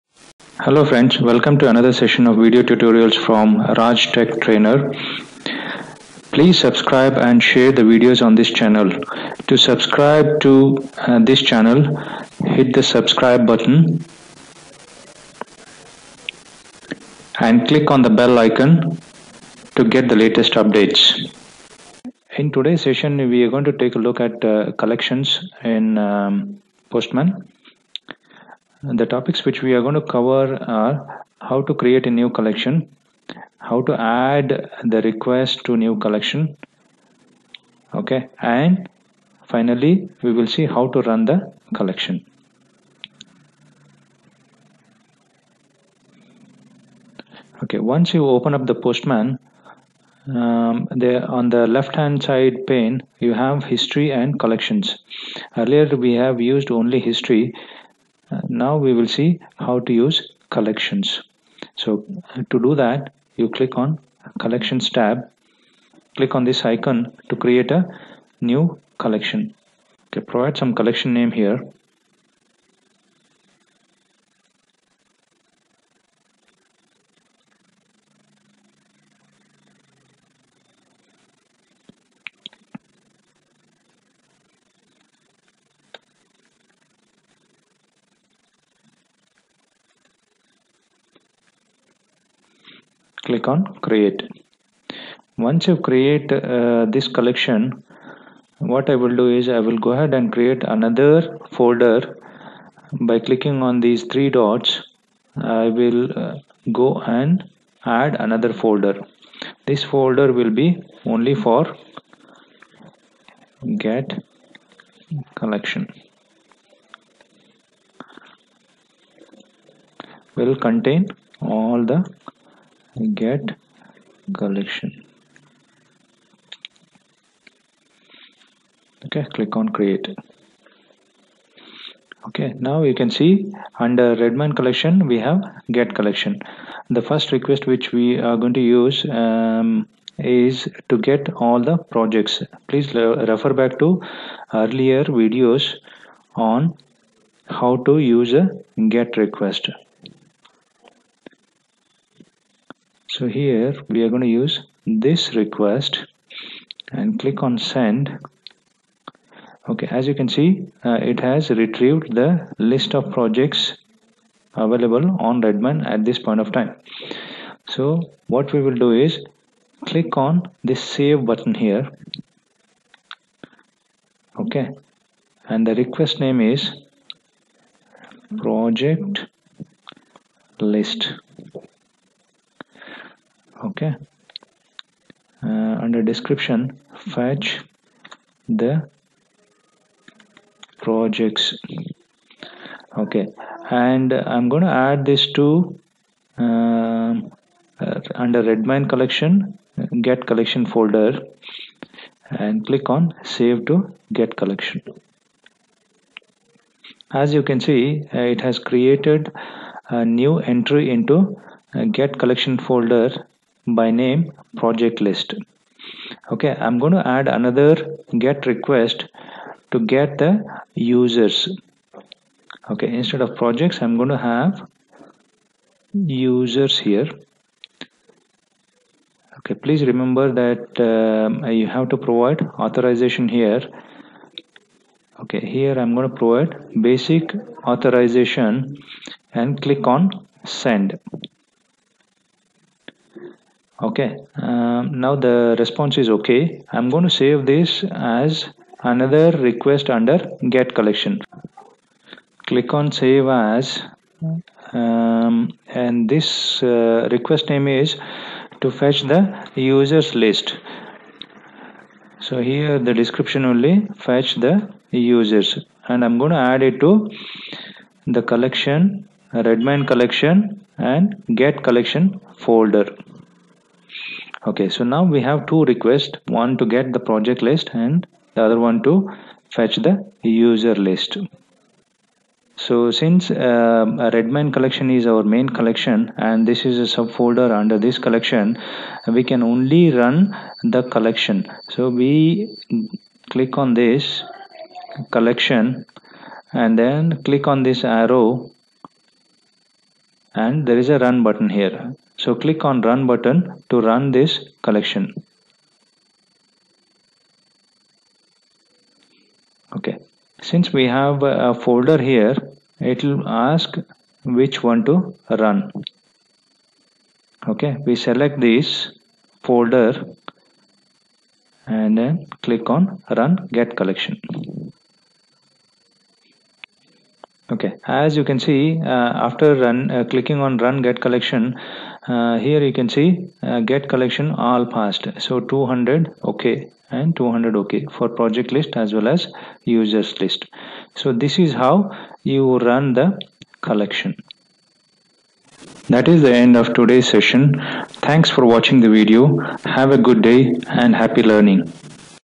Postman Tutorial for Beginners, REST API Testing - Part 5 | Collections | Test API
Topics Covered:
Collections
 Create a new collection
 Add Requests to new collection
 Run the collections